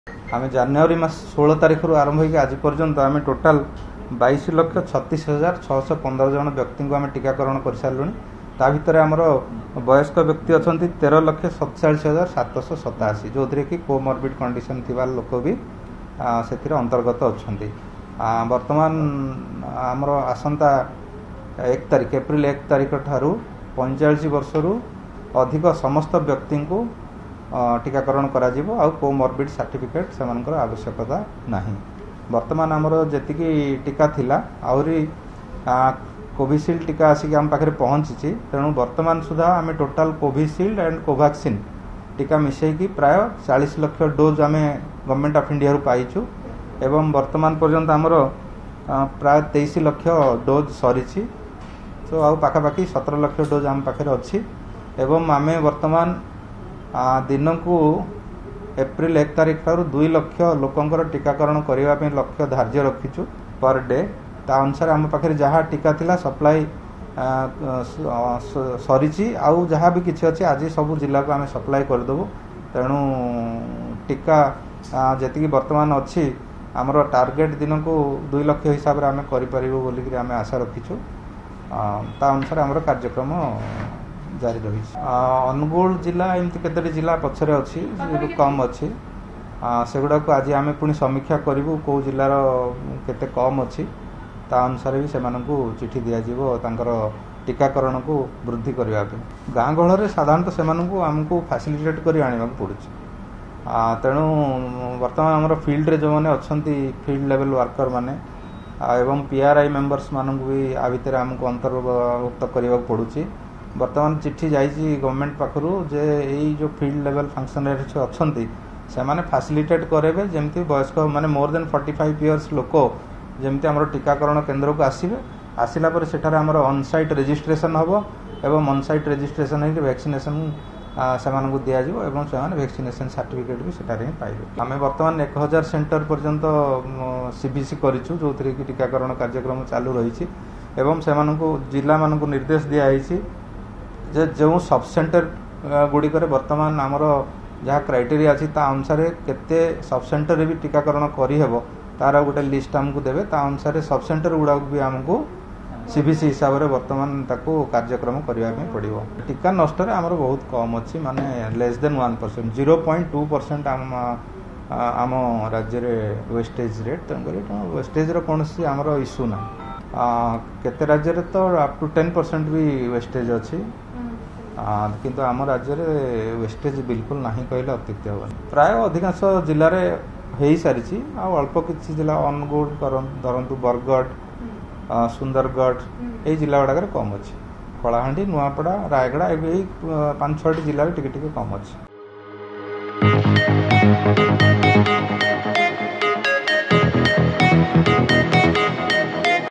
Reaction of Dr. Bijay Panigrahi about Covid Vaccination in Odisha | Odisha Reporter
ରାଜ୍ୟରେ କରୋନା ଟିକାକରଣ ଠିକ୍ ଭାବରେ ଚାଲିଛି ଏବଂ ଟିକାକରଣ ଆରମ୍ଭ ହେବା ଦିନଠାରୁ ରାଜ୍ୟରେ 22ଲକ୍ଷ 36ହଜାର 615ଜଣ ଟିକା ନେଇ ସାରିଲେଣି, ତେବେ ଆଗକୁ ଟିକାକରଣ ପ୍ରକ୍ରୀୟା ଠିକ୍ ରହିବ ବୋଲି କହିଛନ୍ତି ଡାକ୍ତର ବିଜୟ ପାଣିଗ୍ରାହୀ